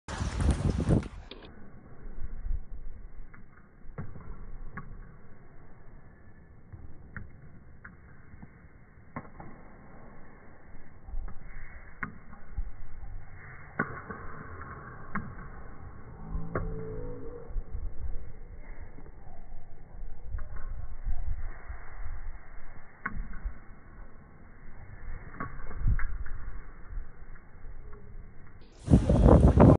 ยางจีน ปะทะ ยางเม็ด
ลูกแรกตบได้ ลูกสอง ตัดมา แทบจะหยุดอยู่กับที่ หน้าไม้เลยปิด  ติดเนต ซะงั้น